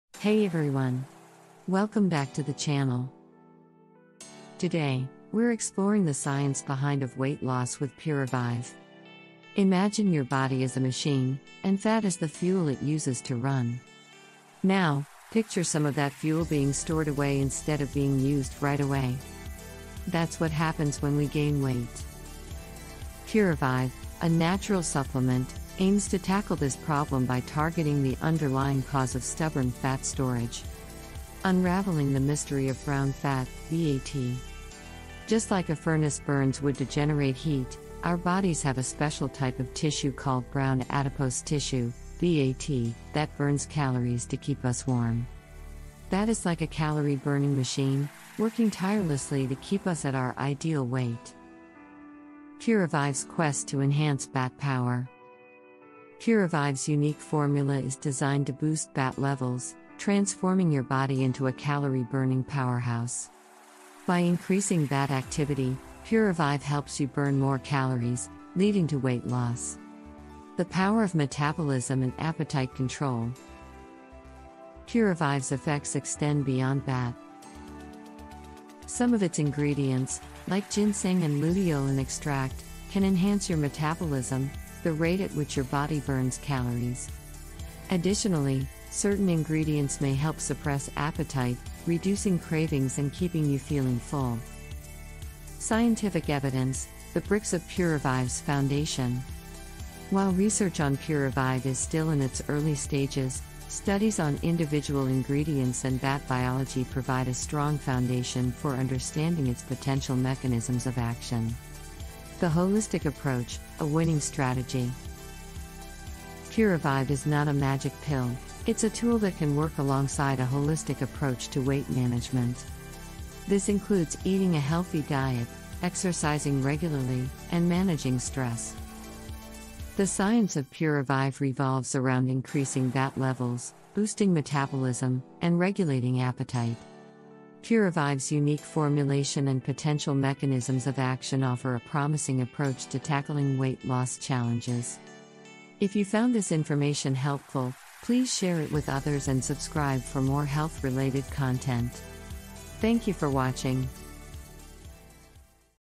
THE SCIENCE BEHIND PURAVIVE WEIGHT LOSS PILLS – PURAVIVE REVIEW
Imagine your body as a machine, and fat as the fuel it uses to run. Now, picture some of that fuel being stored away instead of being used right away. That's what happens when we gain weight.Puravive Weight Loss Pills , a natural supplement, aims to tackle this problem by targeting the underlying cause of stubborn fat storage.

 CHAPTERS
00:00 Welcome
00:08 Introduction
00:33 Unraveling the mistery of BAT
00:55 How Puravive enhance BAT
01:13 Metabolism and Appetite Control
01:37 Scientific Evidence
01:53 Holistic Approach
02:25 Thank you